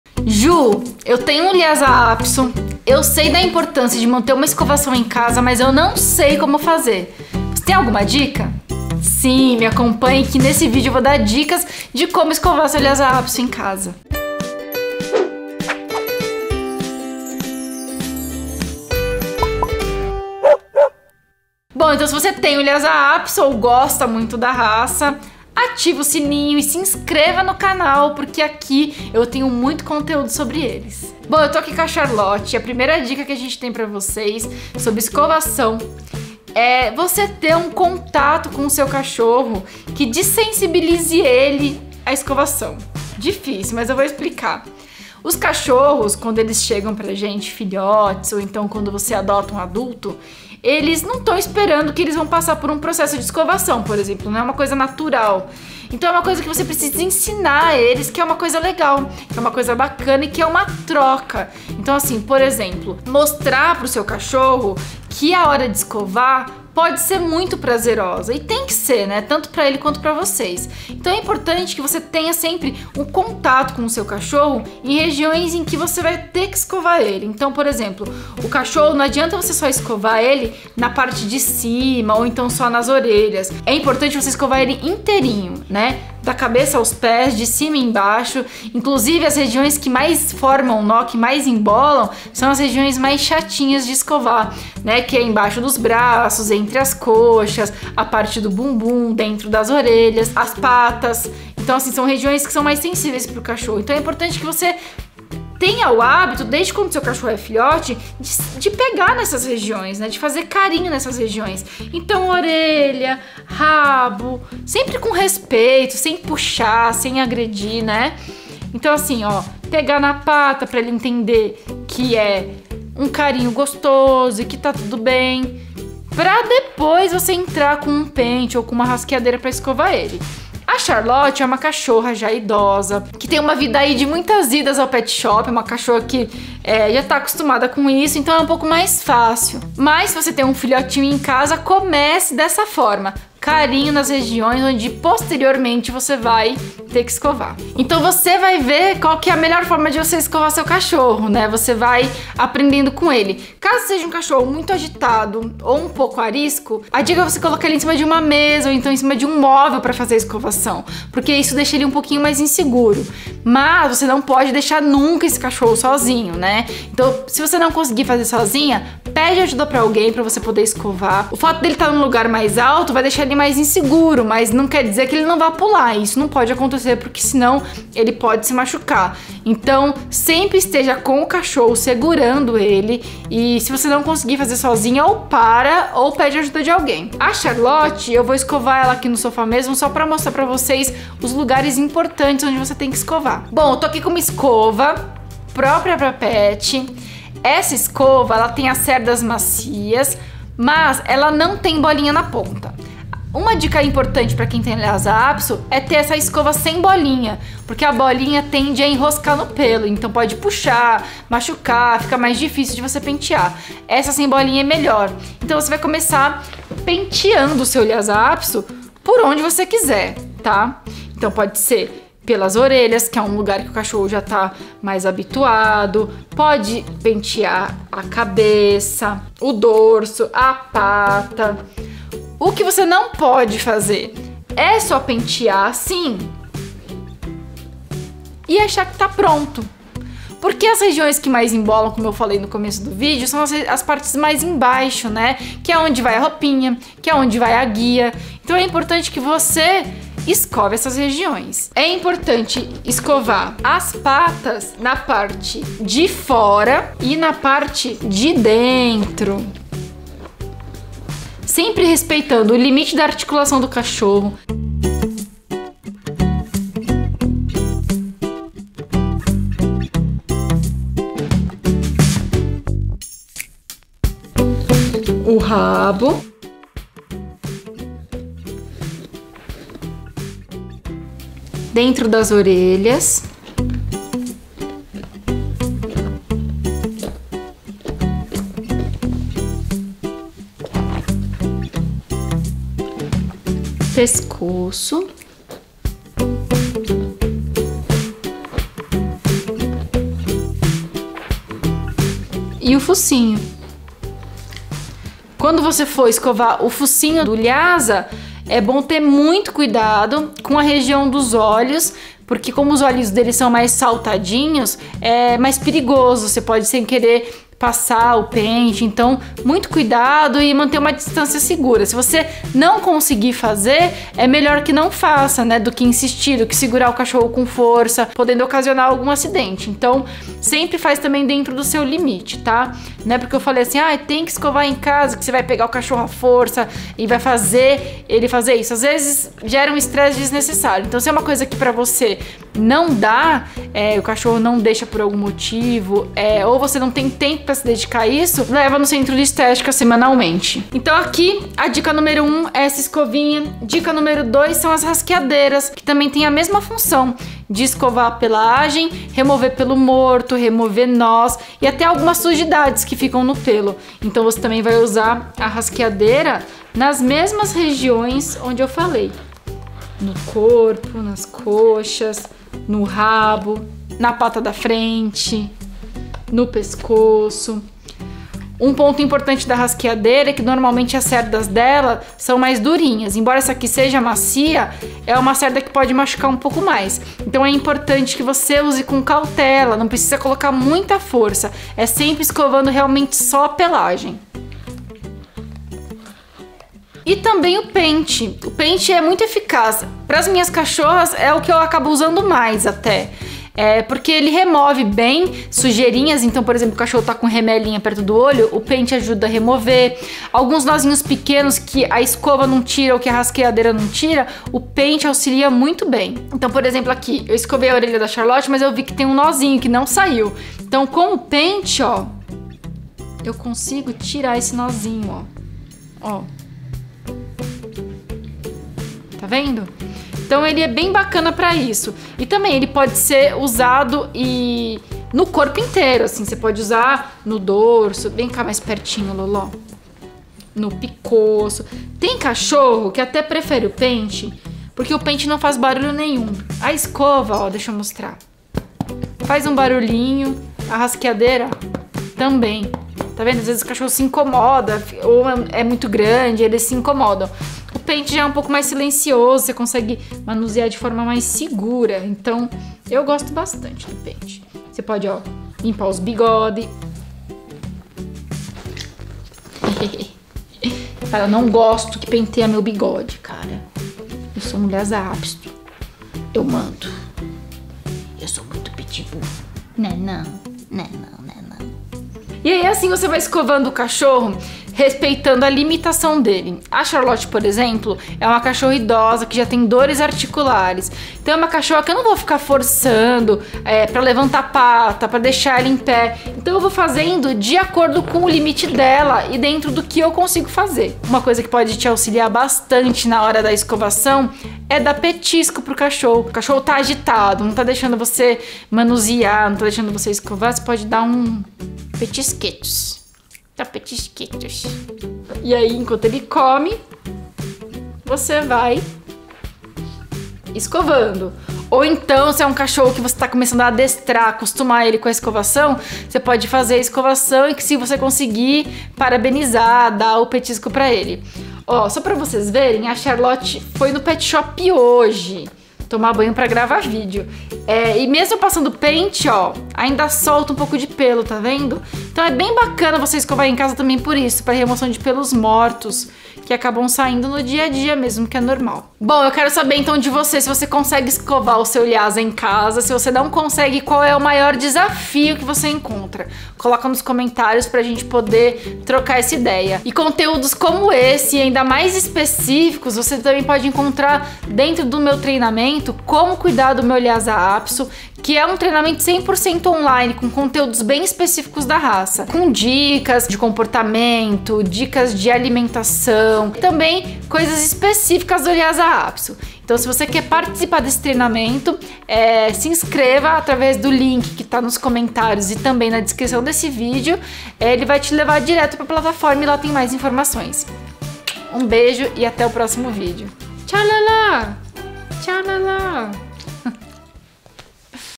Como Escovar os Pelos do Cachorro (Tipos de Escovas) Lhasa Apso
Eii... Vem pro meu Grupo Vip! Vou te ajudar a cuidar do seu Pet... Acesse Nesse vídeo eu falo sobre os cuidados que você precisa ter com a pelagem do Lhasa Apso, como usar e quais são os tipos de escovas necessárias.
Falamos sobre Pente para Cachorro, Escova para Cachorro e Rasqueadeira para Cachorro.

As mesmas técnicas também podem ser utilizadas para outros cães de pelo longo, como por exemplo Shih Tzu, Yorkshire, Maltês entre outros.